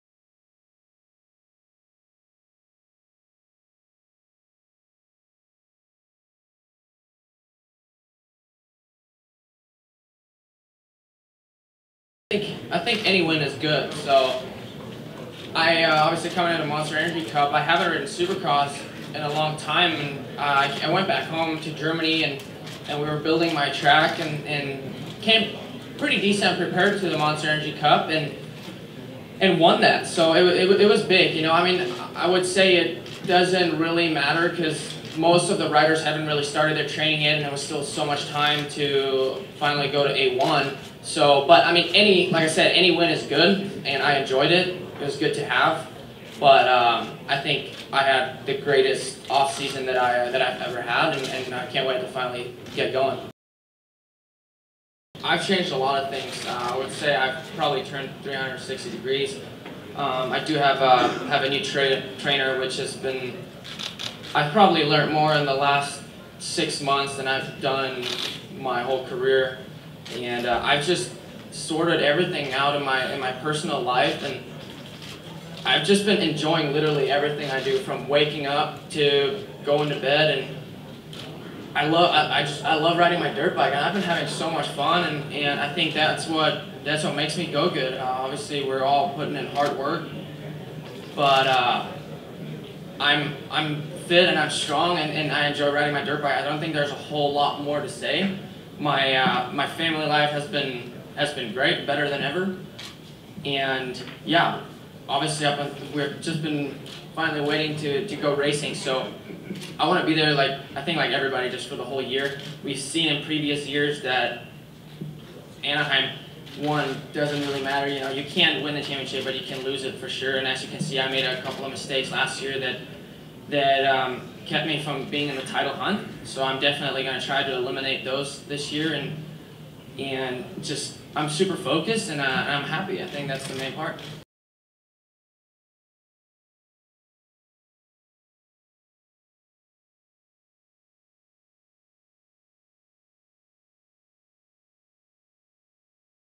Ken Roczen talks about last season and his preparation for 2016
Monster Energy Supercross clips from the Press Conference